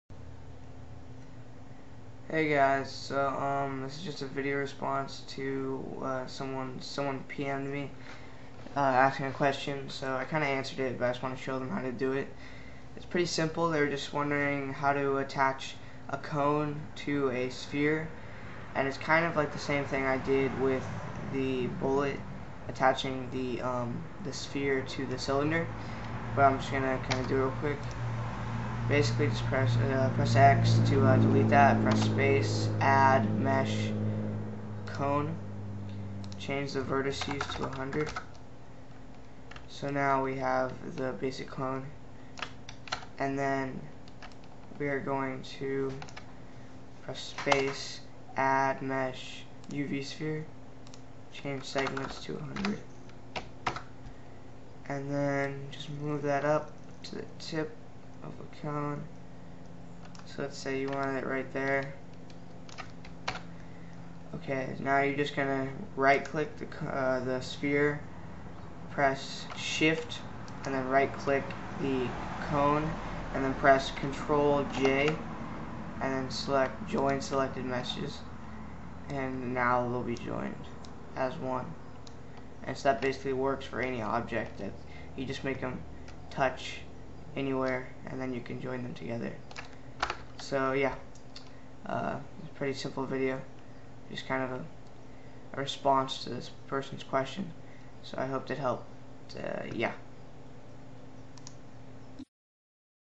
Blender Tutorial - How to Merge Two Objects Together
Hey guys this is just a response to a question I got asking about how to merge together a cone and a sphere.  So if you had any similar questions I hoped this helped.

Thanks.

-Nate